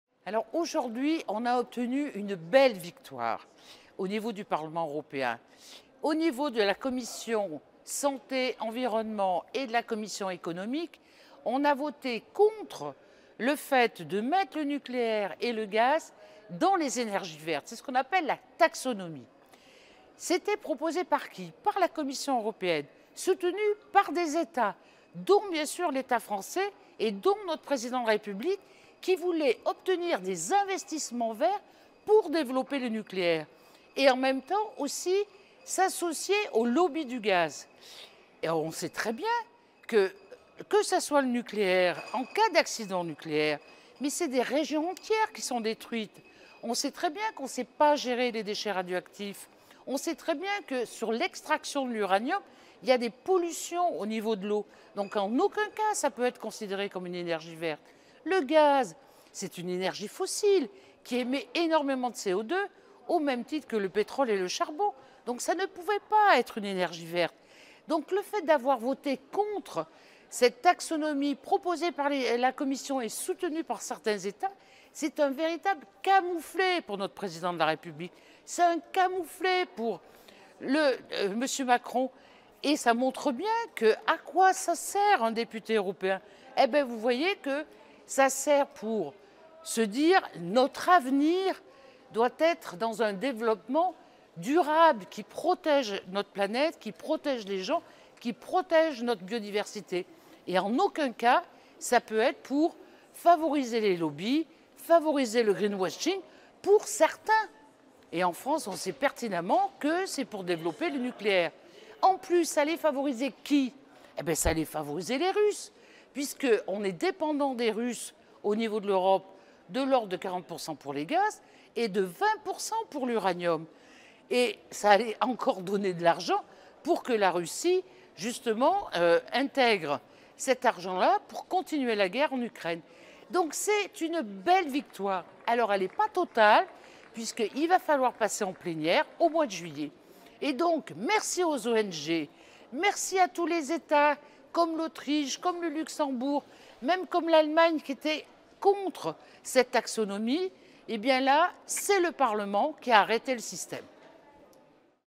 Une première victoire pour exclure le nucléaire et le gaz des investissements verts !
Mardi 14 juin, les députés des commissions de l'environnement, de la santé publique et de la sécurité alimentaire (ENVI) et des affaires économiques et monétaires (Econ) ont voté contre la proposition de la Commission européenne d'inclure le nucléaire et le gaz dans la taxonomie  : 76 pour et 62 contre. Le gaz et le nucléaire ne sont pas verts et ne doivent pas bénéficier d'aides à la transition écologiques ! Rdv en juillet lors du vote final en session plénière.

5 raisons de s'opposer à l'inclusion du gaz et du nucléaire dans la taxonomie